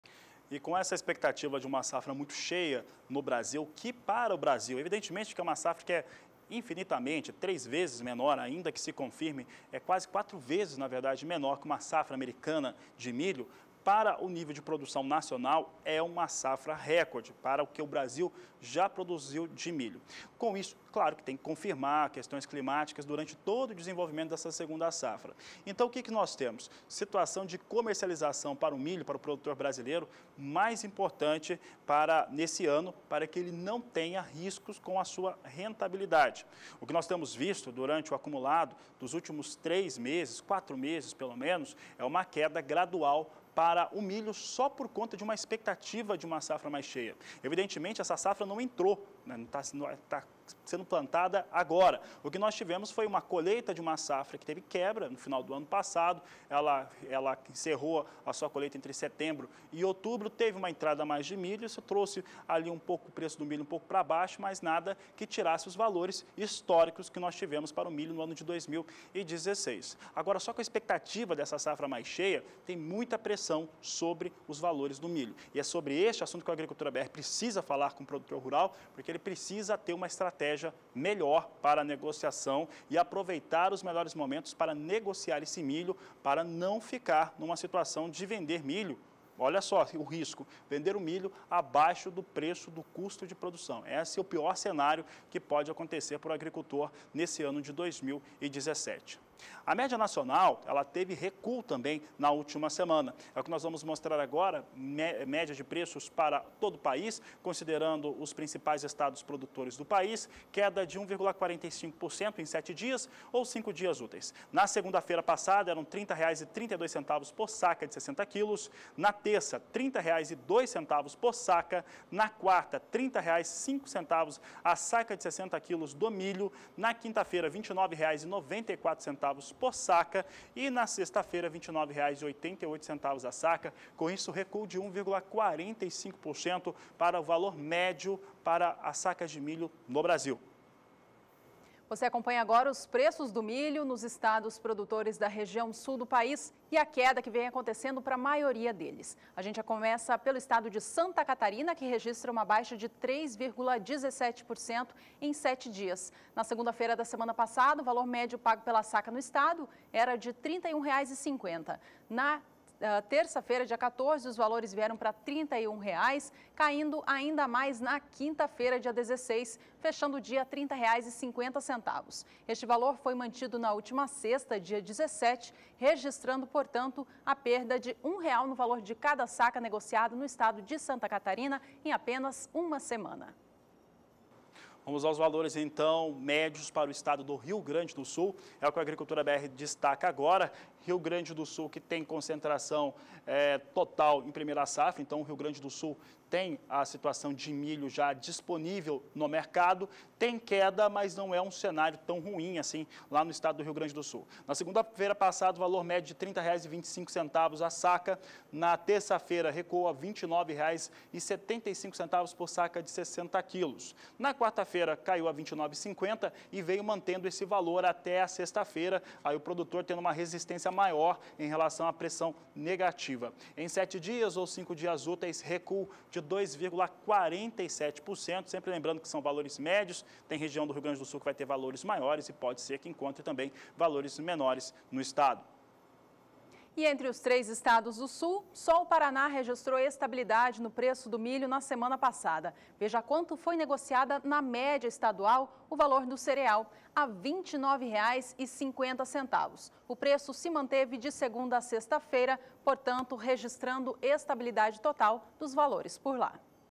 INDICADOR MILHO BR SC RS PR - 20/02/2017
Acompanhe a média de preço do milho 7 dias praticado no Brasil e nos estados de Santa Catarina, Rio Grande do Sul e Paraná.